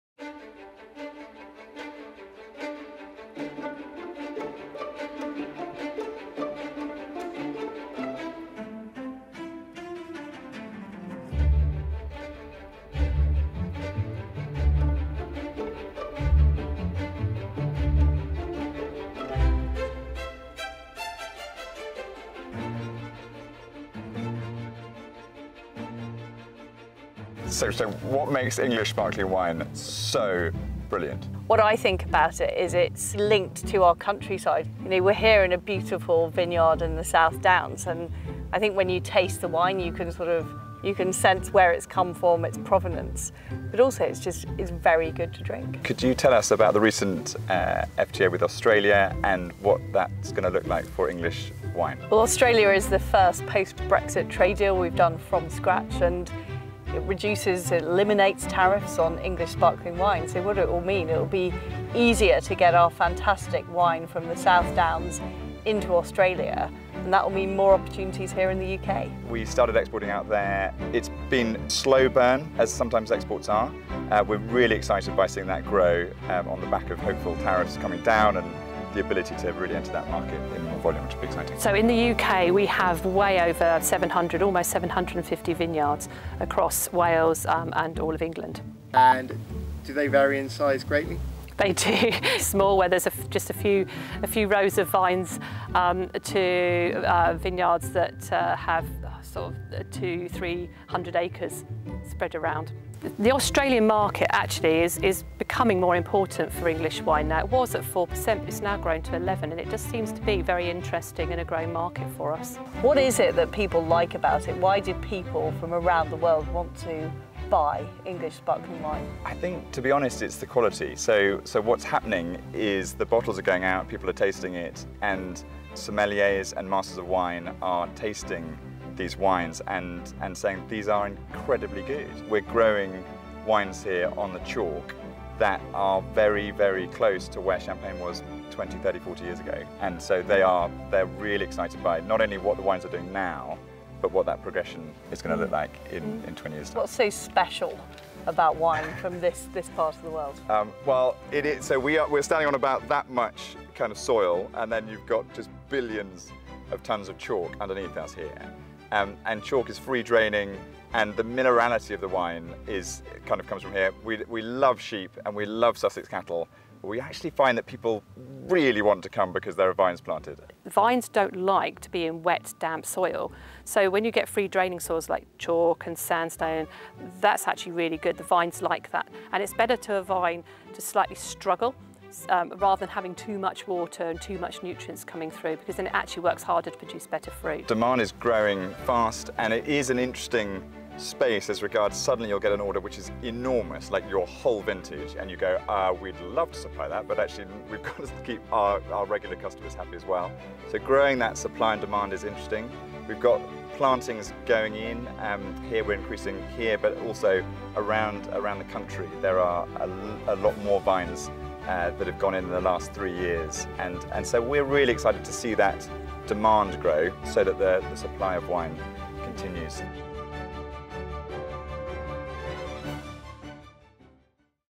How an FTA between the UK and Australia helps wine exports
In this video, Secretary of State for International Trade Liz Truss visits the Wiston Estate vineyards, near Pulborough in West Sussex, one of the UK's top exporters of English sparkling wine.

Speakers:

- Liz Truss, Secretary of State for International Trade
- Richard Goring, Managing Director - Wiston Estate Winery.
- Sam Linter, Director - WineGB

To find out how DIT can help your business export,